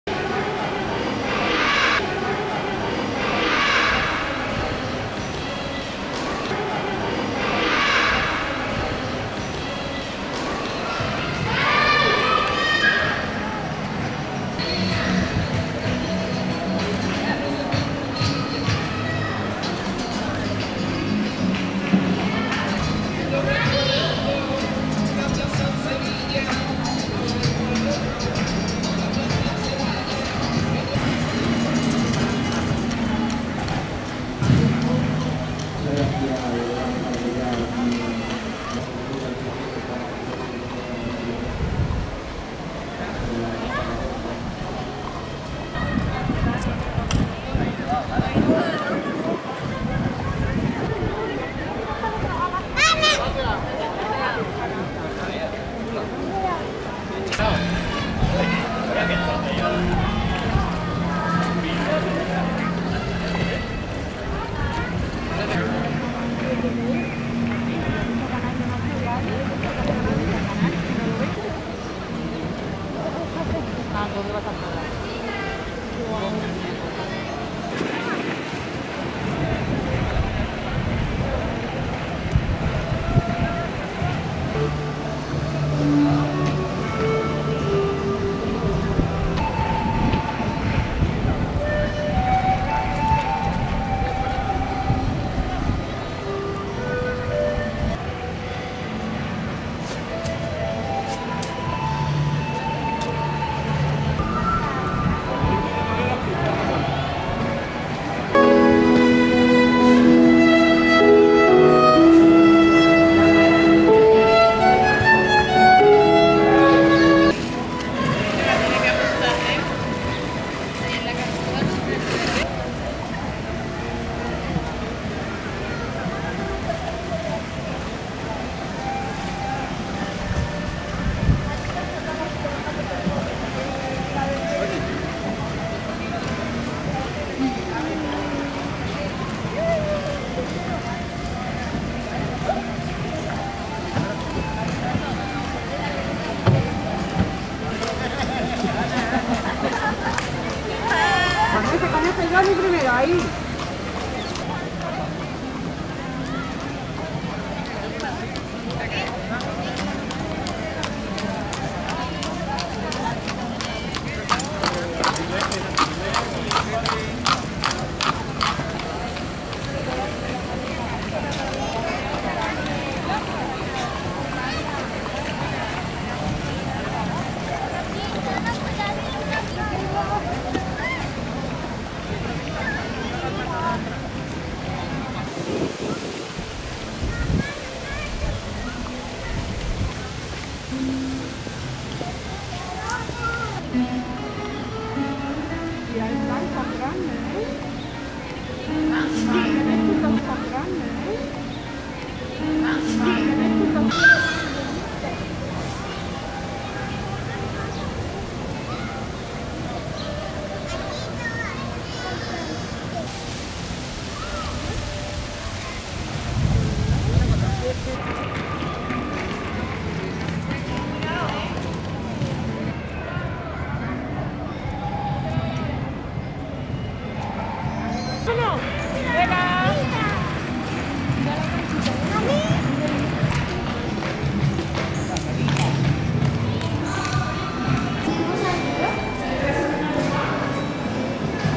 PLAZA DE ESPAÑA 25 OCTUBRE 2020 SEVILLA 4K
PLANOS DE LA PLAZA DE ESPAÑA DE SEVILLA CON LA Sony Handycam FDR-AX53

PLANOS Y AUDIOS SIN EDITAR.